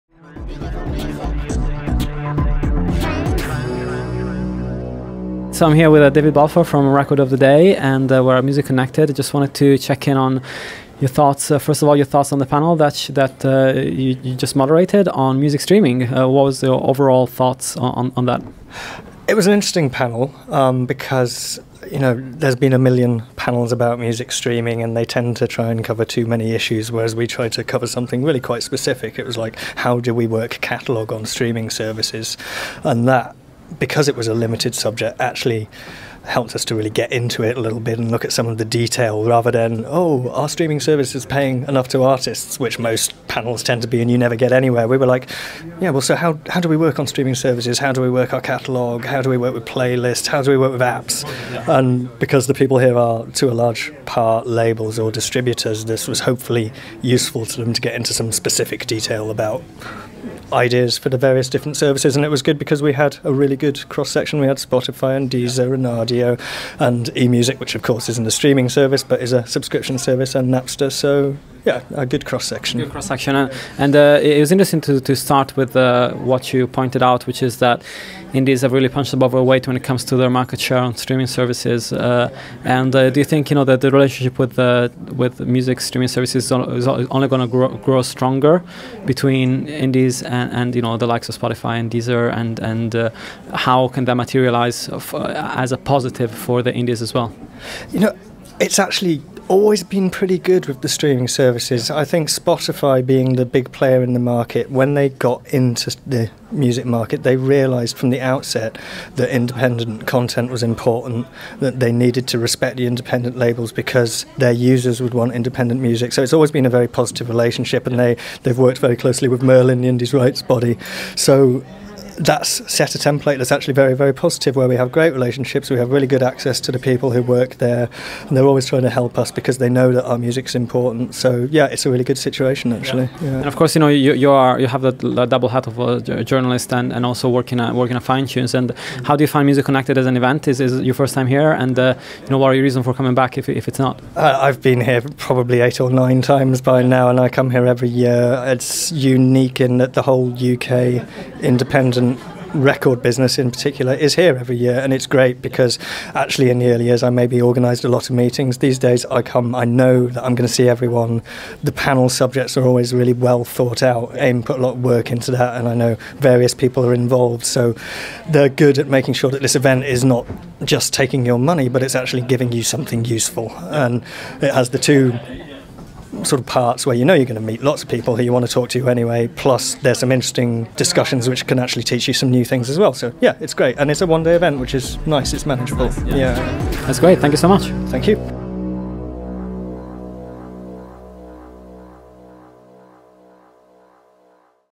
David Balfour, ROTD - DMT at Music Connected 2013
Digital Music Trends chats with David Balfour about the panel he moderated on increasing exposure of one's catalogue on digital services, which included representatives from most of today's largest on-demand streaming companies.